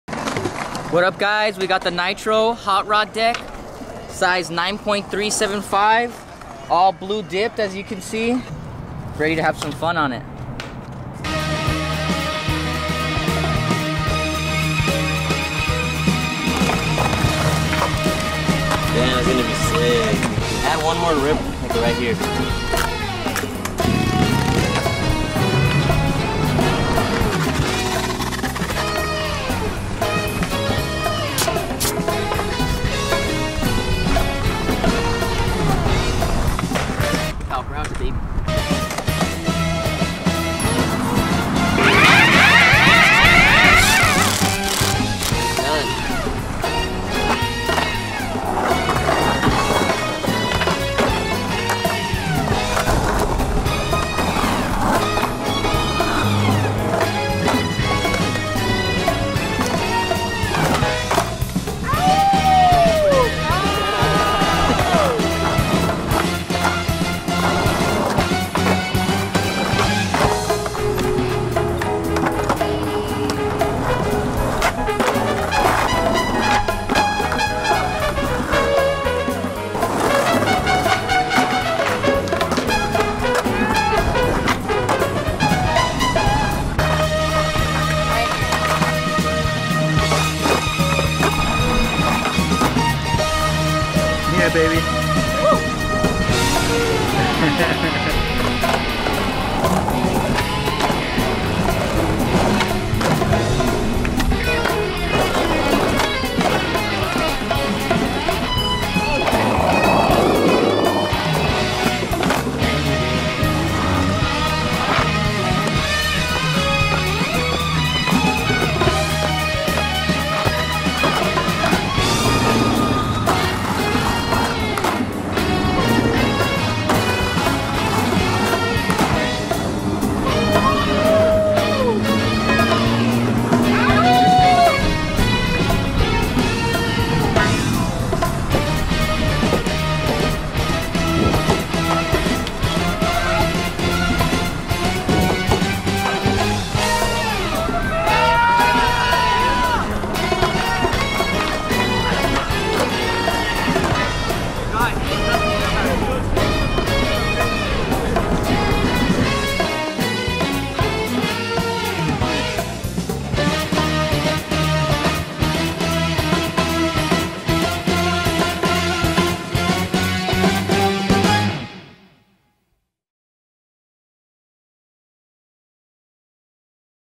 NITRO Hot Rod Flames Skateboard
NITRO Hot Rod Flames skateboard, all set up!
Join on the parking lot session with Zach Doelling, Cole Perera, Chris Hiett, Orlando Garcia, Cy Romano, and Donny Hixson.

Filmed by Deville.